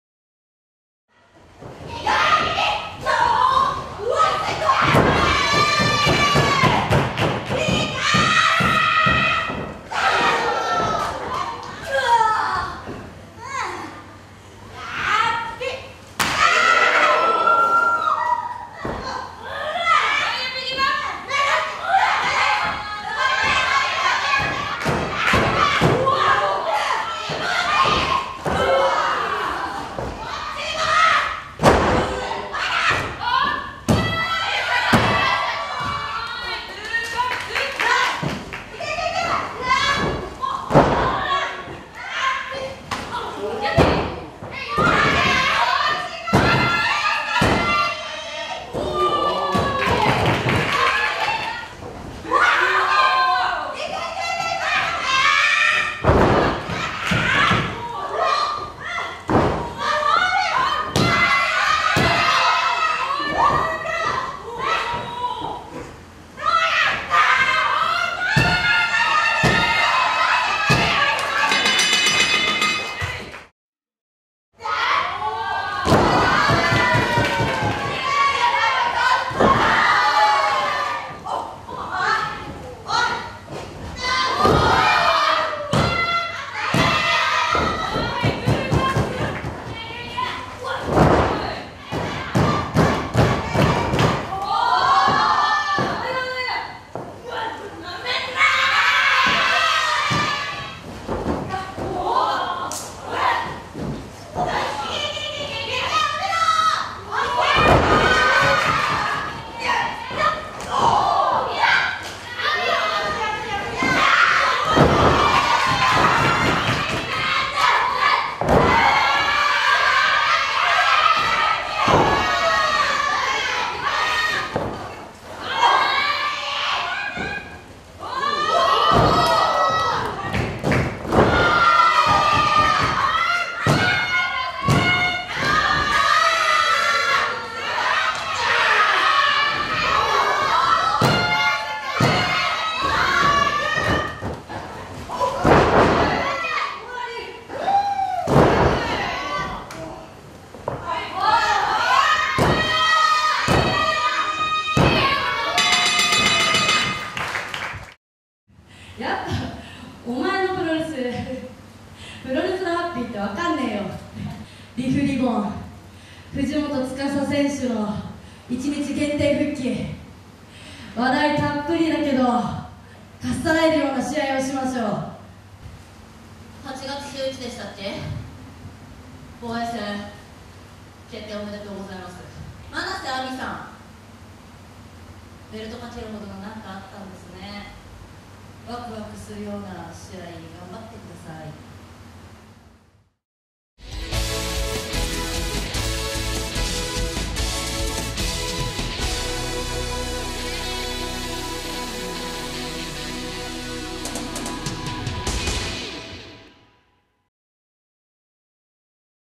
ICERIBBON 1290(2023/7/22)Digest
2023年7月22日に行われたアイスリボン1290の大会ダイジェスト。

8.26後楽園ホールでリボンタッグへの挑戦が決まったYappyが、前王者星いぶきとシングルマッチ。

7.30利府リボンで、ノンタイトルでのシングルマッチを行うYuuRIと海乃月雫。YuuRIは杏ちゃむと、海乃はアイスリボン初参戦のミア・フライディとのタッグで対戦。

アイスリボンの直近大会スケジュール